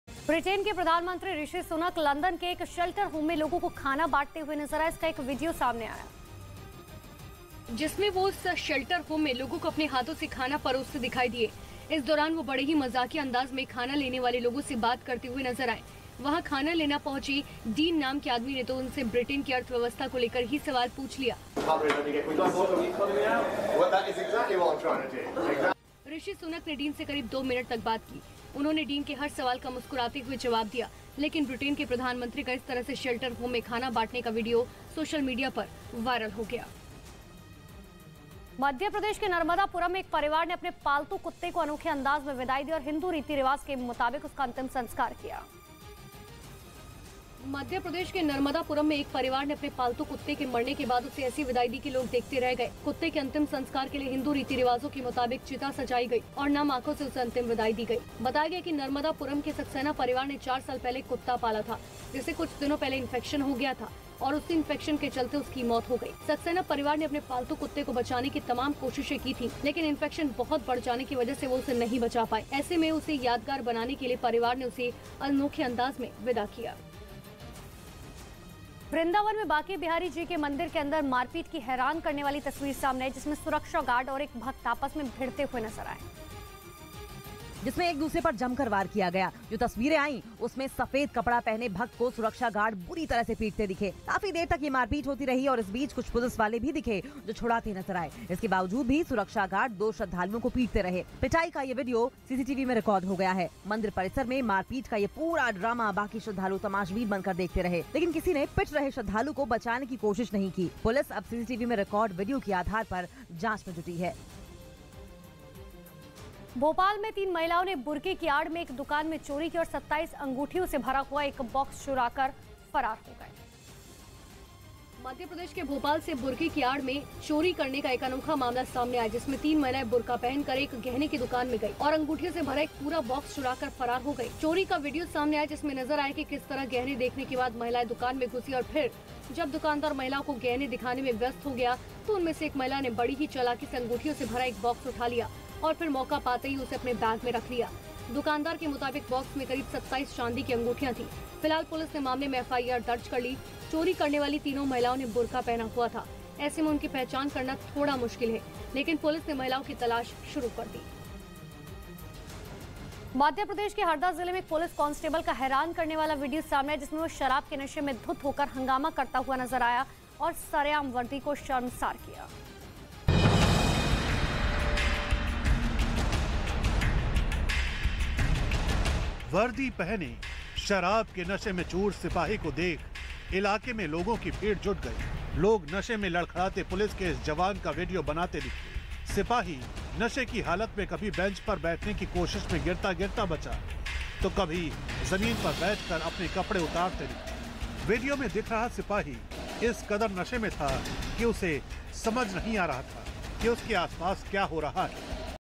Rishi Sunak: Britan के PM का विडियो हुआ वायरल, लोगों को खाना बांटते दिखे ऋषि सुनक | Hindi News
Rishi Sunak: Britan के PM ऋषि सुनक  London के एक Shelter home में लोगों को खाना बांटते हुए नज़र आए। विडियो हुआ वायरल।